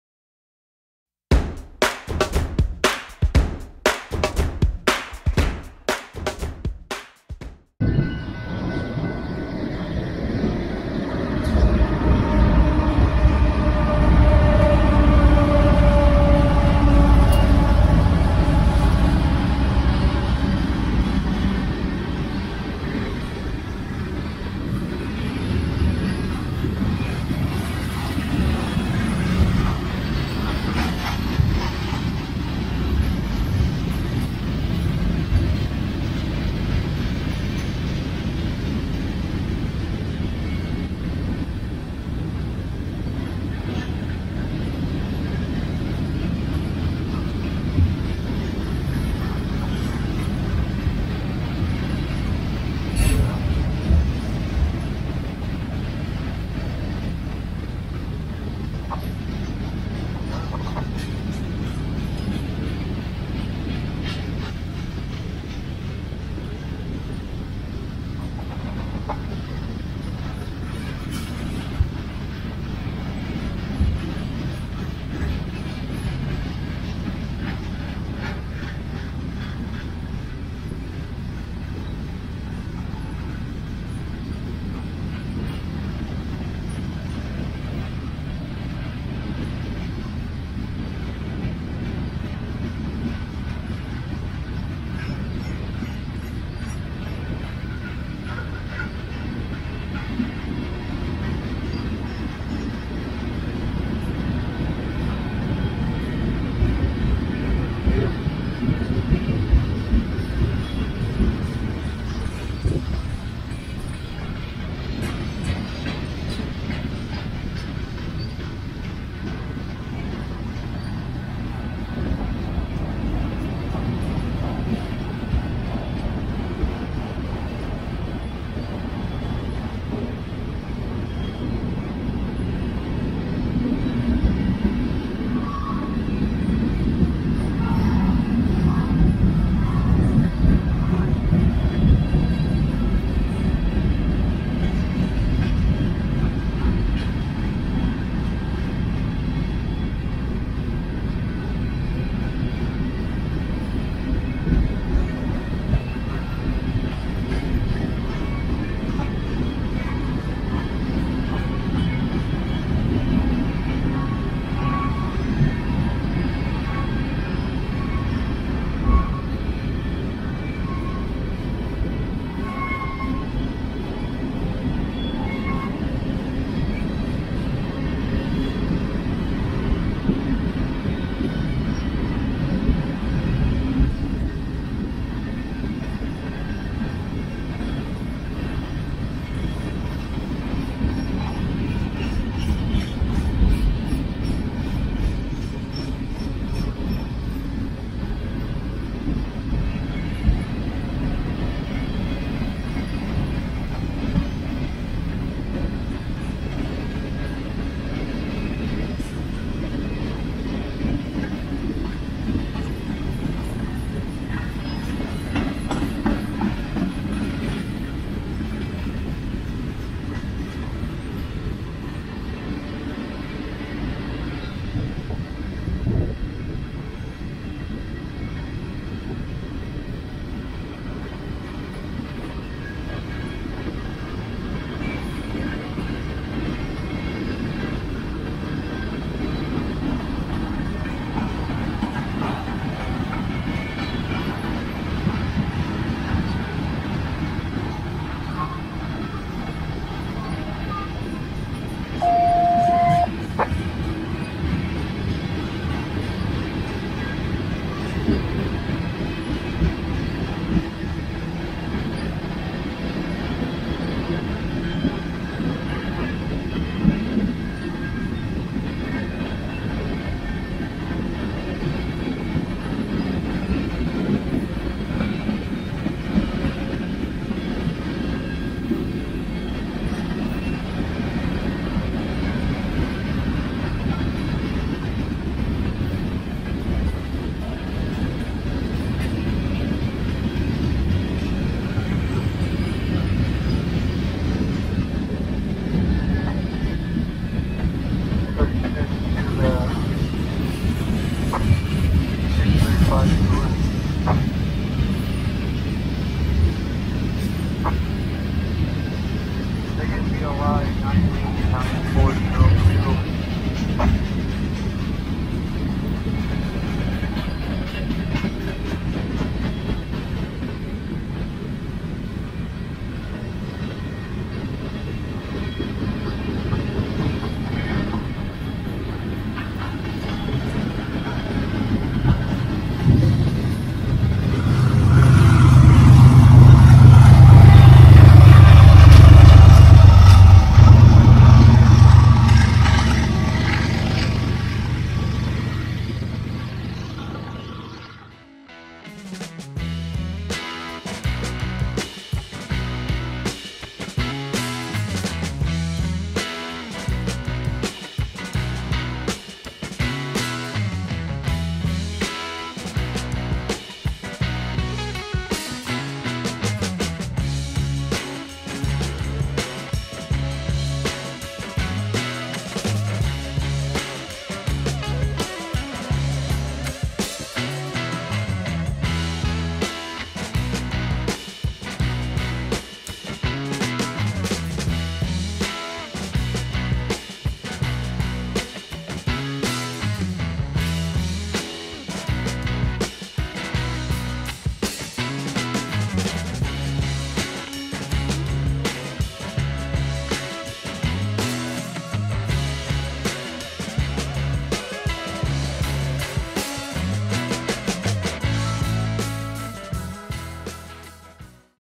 A Dark & Cloudy Afternoon Manifest
Not the best lighting in this video but a northbound BNSF manifest on the Galveston Sub in Caldwell.